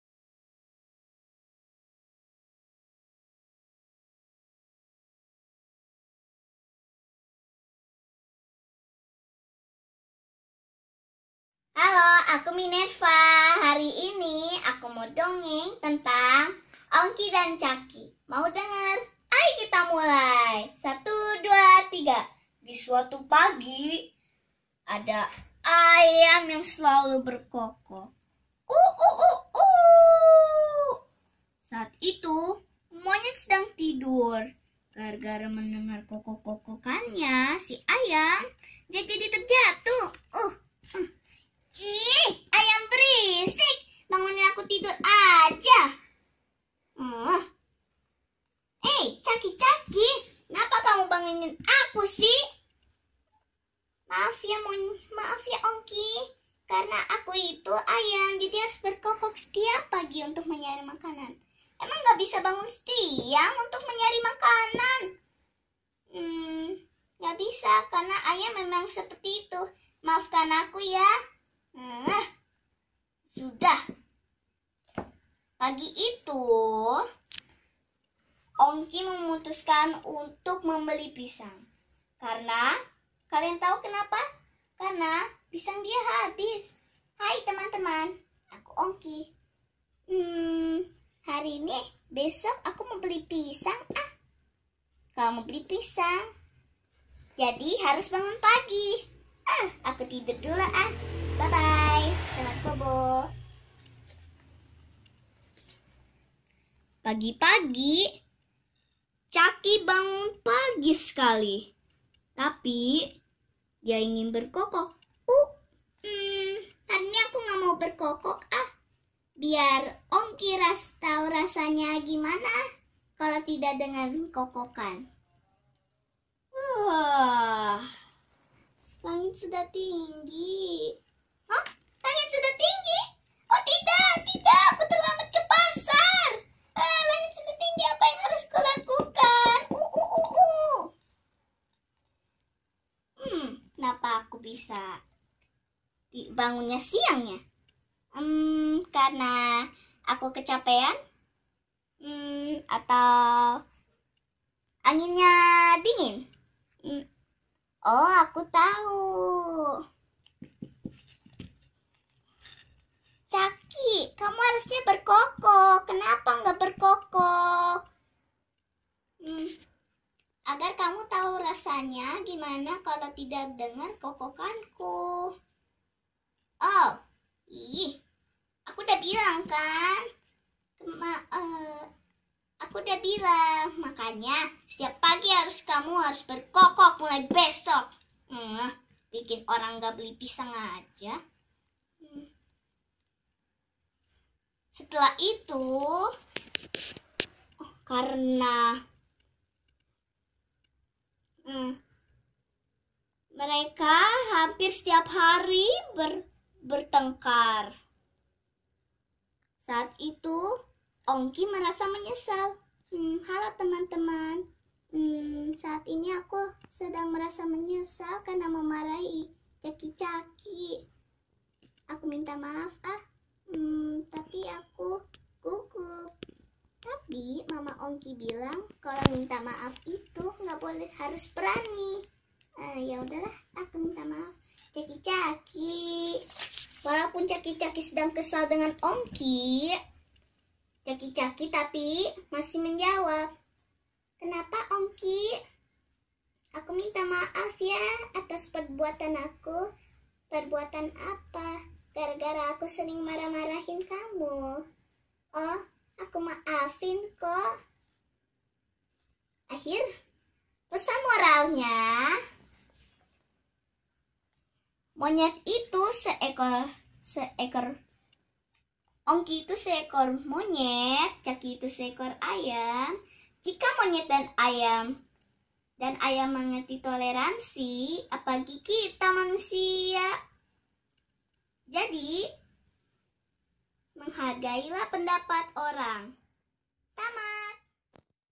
RUMAH PINTAR BSD CITY : SENTRA BERMAIN (Dongeng tentang Toleransi)
Dongeng tentang Toleransi
Judul : Ongki dan Caki belajar toleransi
Karangan dan didongengkan oleh : Minerva Demary, usia 7 tahun